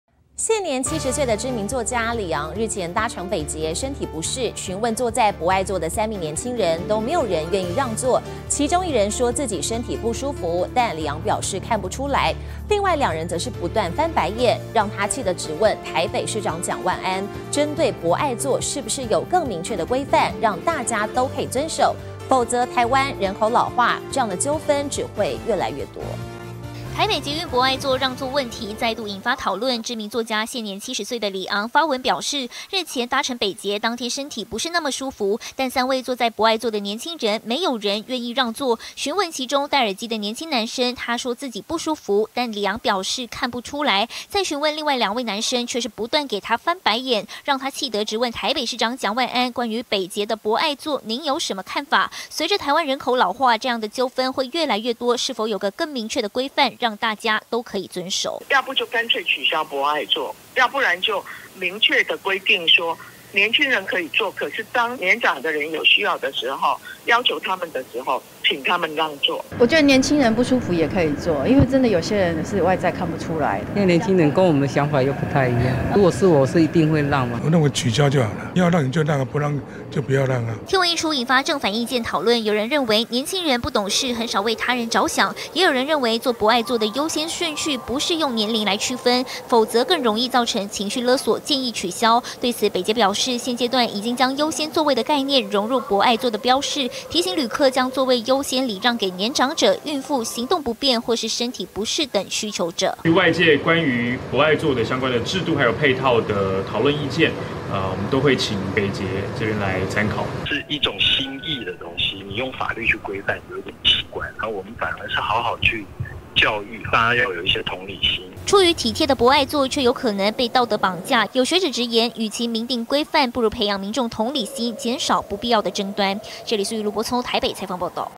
「博愛座如何使用」 李昂點名蔣萬安給回應
現年70歲的 知名作家 李昂， 日前搭乘北捷，身體不適，詢問坐在博愛座的三名年輕人，都沒有人願意讓座，其中一人說自己身體不舒服，但李昂表示看不出來，而另外兩人則是不斷給她 翻白眼，讓她氣得 質問 台北市長 蔣萬安，針對博愛座，是否有個 更明確規範，讓大家 都可以遵守？否則台灣人口老化，這樣的糾紛，會越來越多。